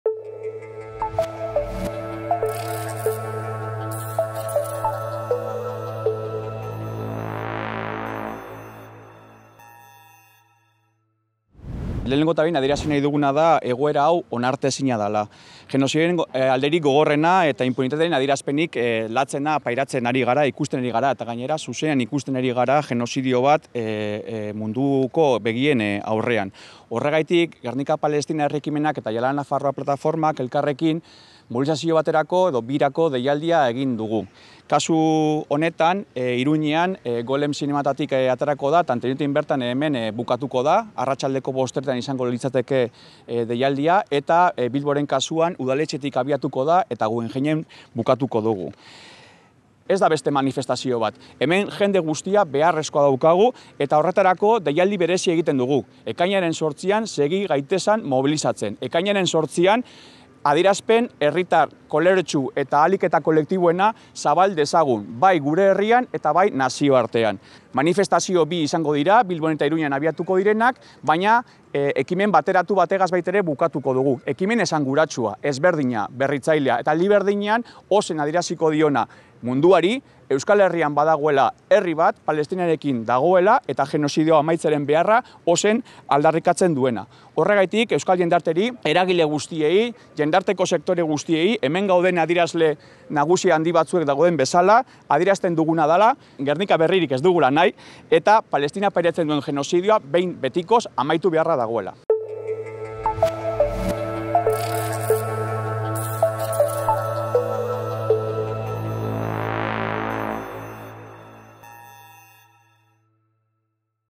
Europako hauteskundeen bezperan Palestinan genozidio gelditzeko aldarria kalera aterako du
Iruñeko manifestazioa larunbat honetan 17:30etan abiatuko da Golem zinemetatik eta Antoniutin amaituko da. Euskal gizarteari, eragileei eta gizarteko sektore guztiei genozidioarekin amaitzeko manifestatzeko deia egin diete.